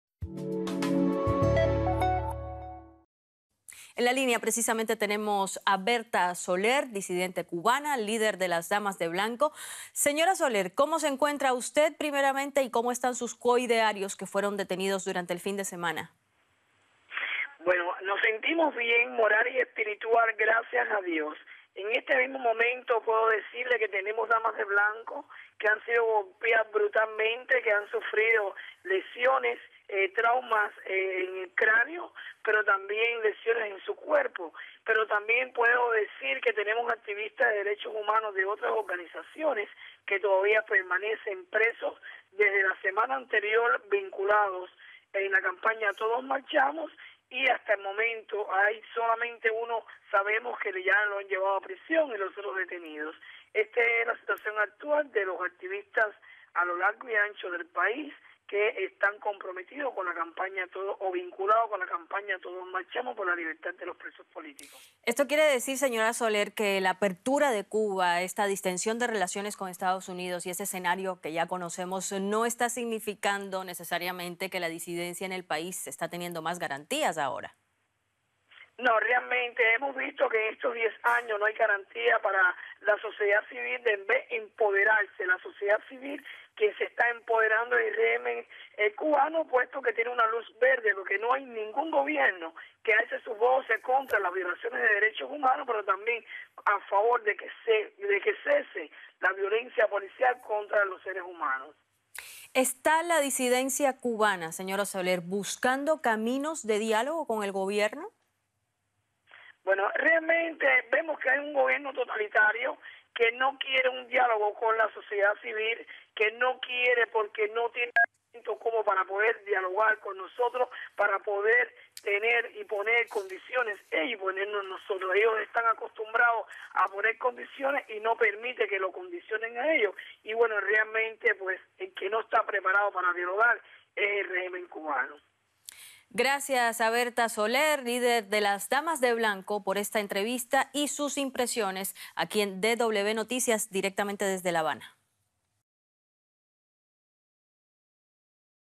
Bertha Soler explica la situación de los disidentes cubanos detenidos
La líder de las Damas de Blanco denuncia violencia contra activistas que piden la libertad de los presos políticos